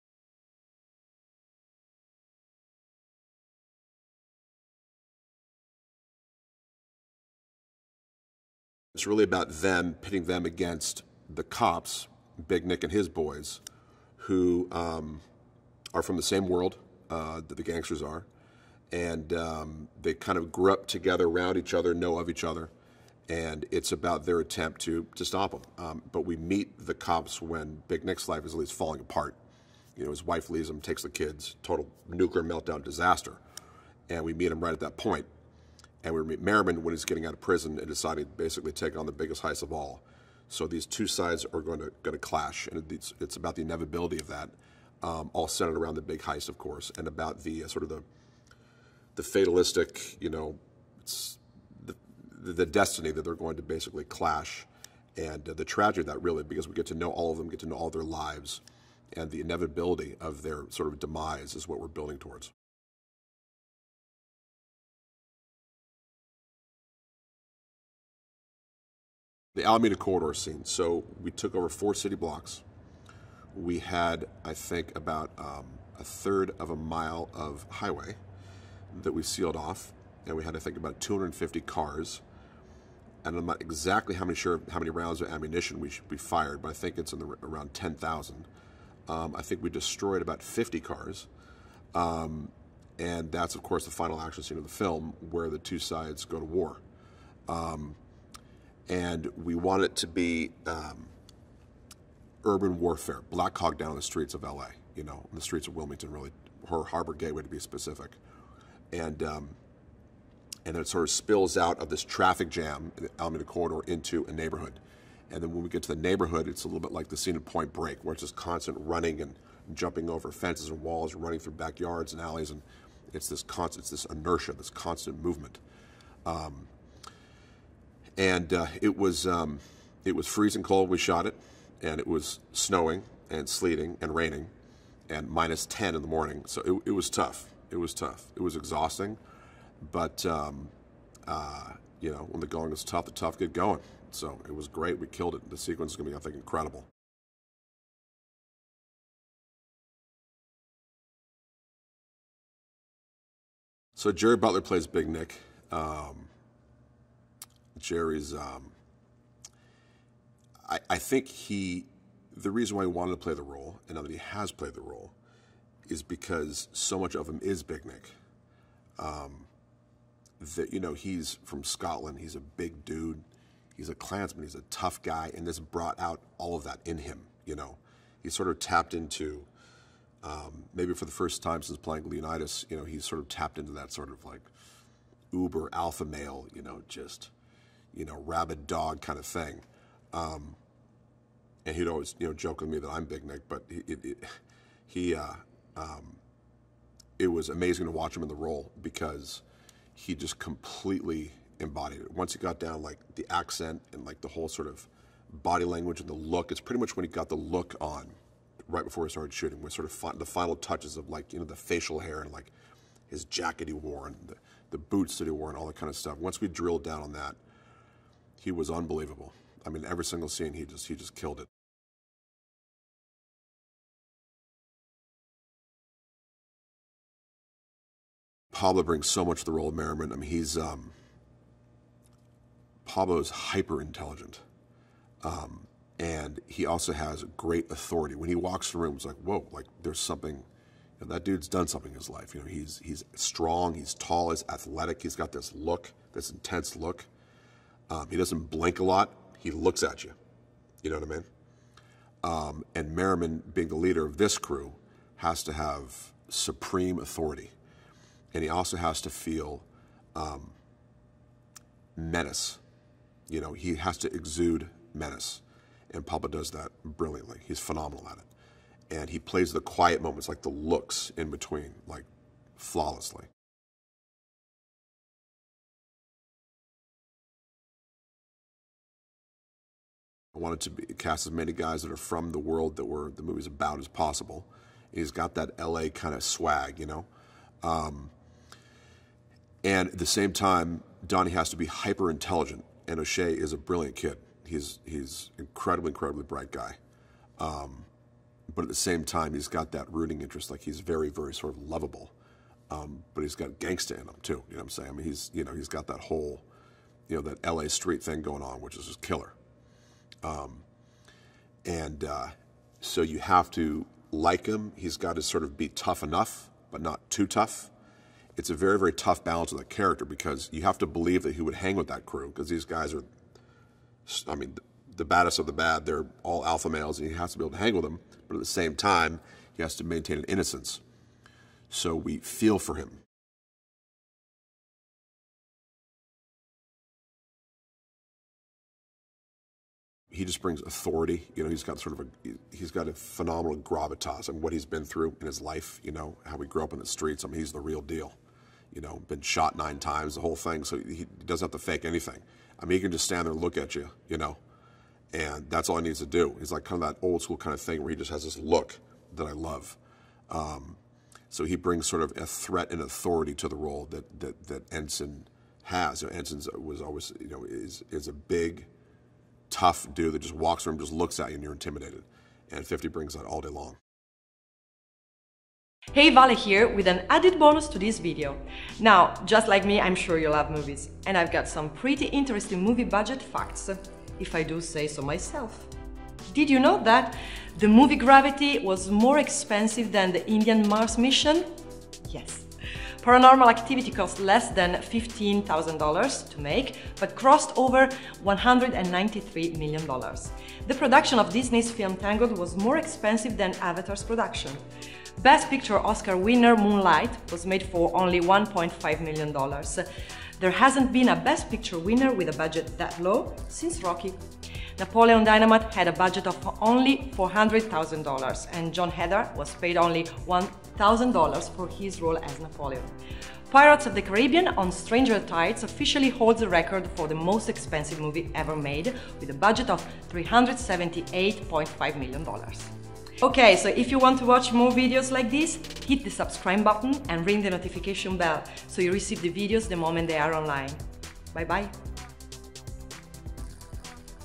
Den Of Thieves | On-set visit with Christian Gudegast "Director"
Den Of Thieves - Christian Gudegast "Director" [On-Set Interview]

A gritty crime saga which follows the lives of an elite unit of the LA County Sheriff's Dept. and the state's most successful bank robbery crew as the outlaws plan a seemingly impossible heist on the Federal Reserve Bank.